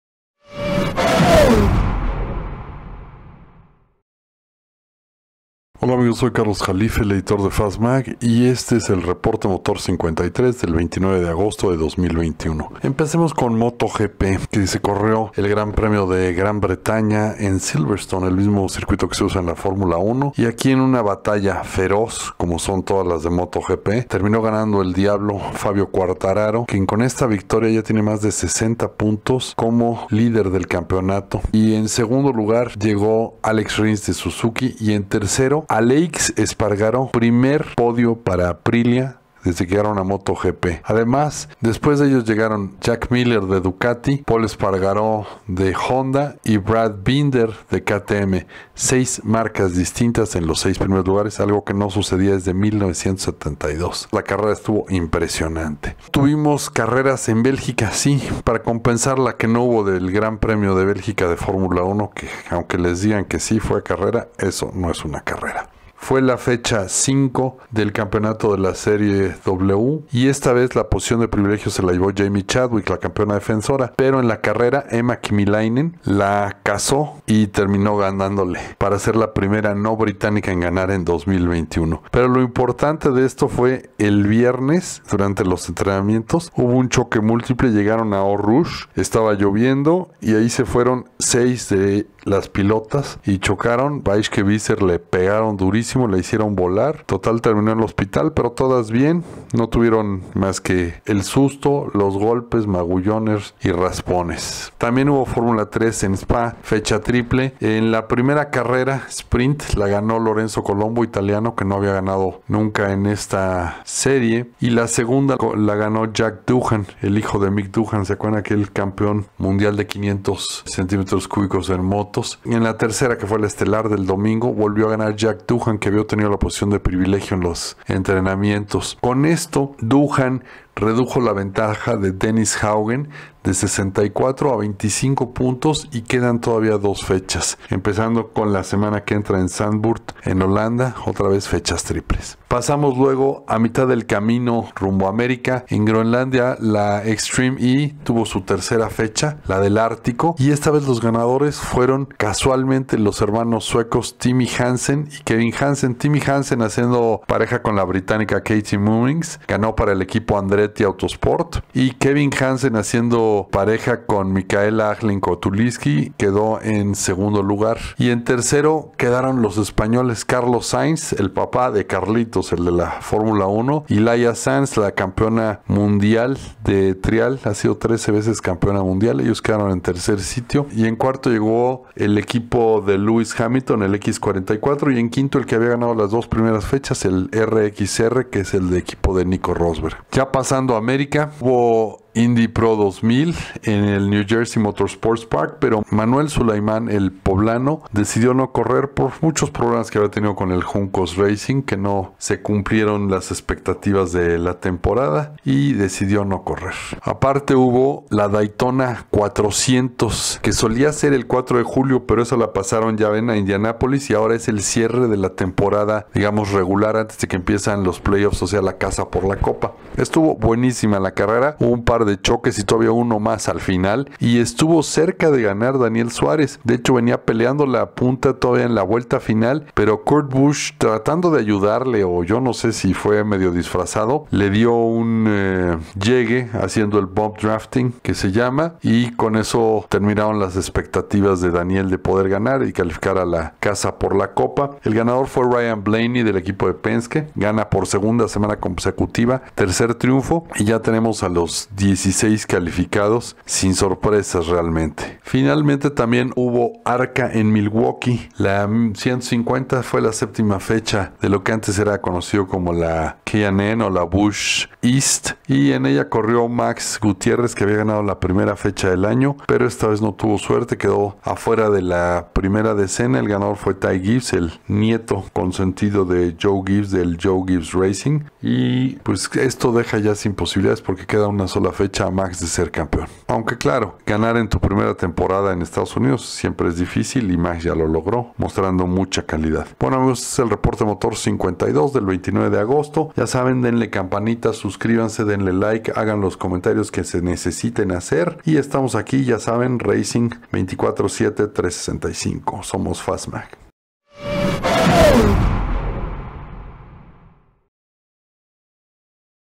¡Listos los playoffs de NASCAR! - REPORTE MOTOR Ep. 53
Copa NASCAR finalizó su temporada regular con una carrera dramática en Daytona, donde DANIEL SUÁREZ estuvo en posición de ganar en la última vuelta.

CARLOS JALIFE te cuenta lo que pasó durante el fin de semana, que incluyó otra clase de manejo de FABIO QUARTARARO en MotoGP, la Fórmula 3 y SERIE W en Bélgica, el X-PRIX DEL ÁRTICO de Extrema E, ARCA con MAX GUTIÉRREZ e INDY PRO 2000 en Nueva Jersey.